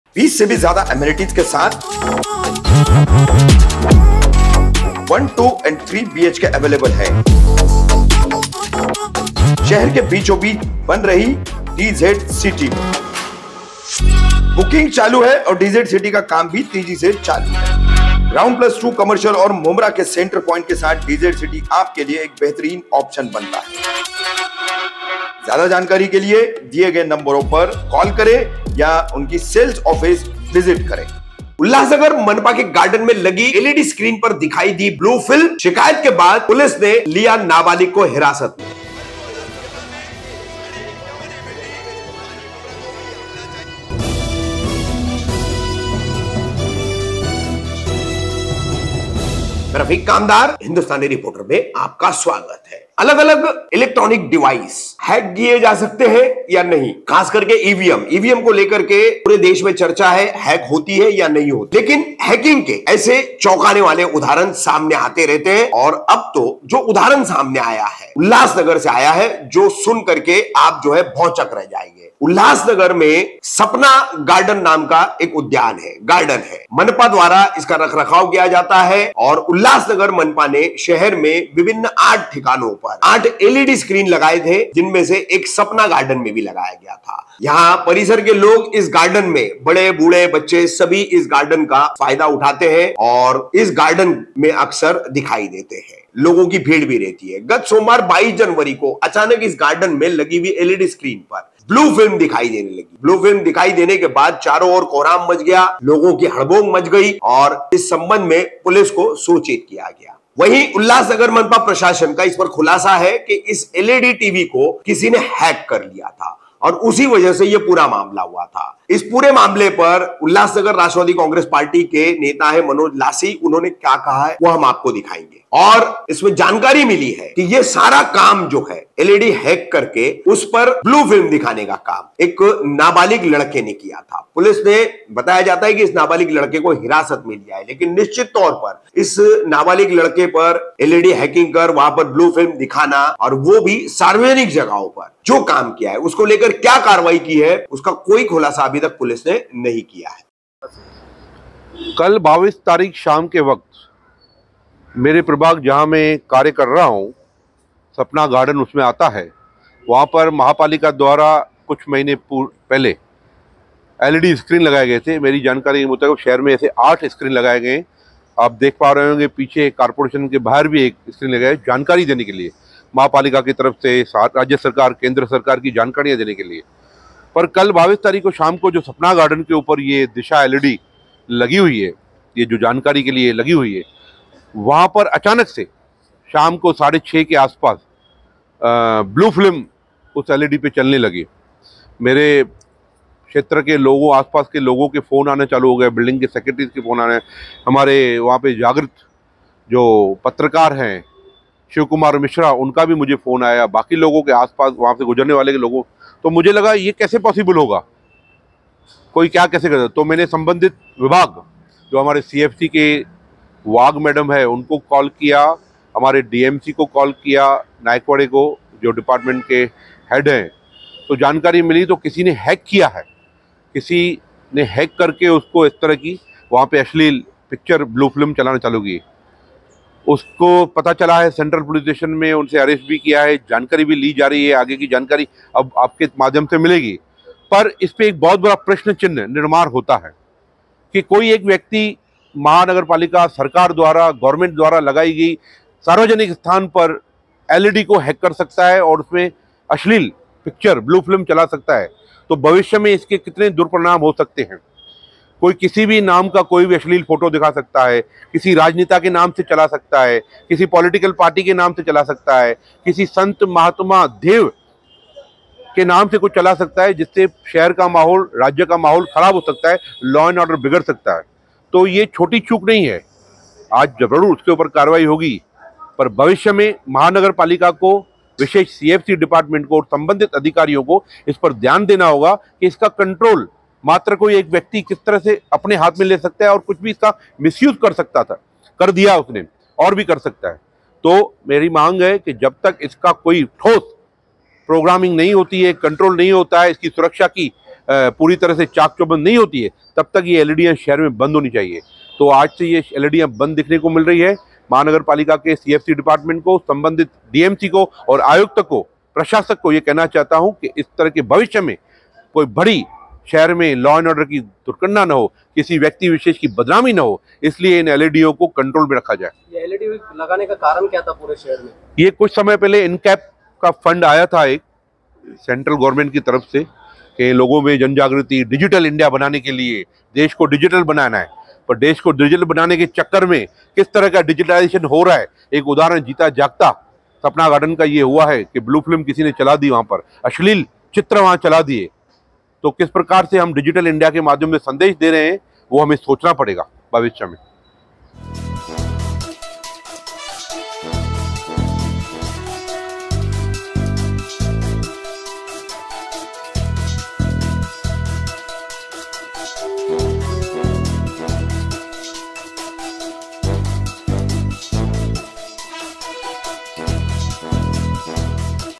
उल्हासनगर मनपा के गार्डन में लगी एलईडी पर दिखाई दी ब्लूफिल्म| Hindustani Reporter
उल्हासनगर मनपा के गार्डन में लगी एलईडी पर दिखाई दी ब्लूफिल्म

पुलिस ने लिया नाबालिग को हिरासत में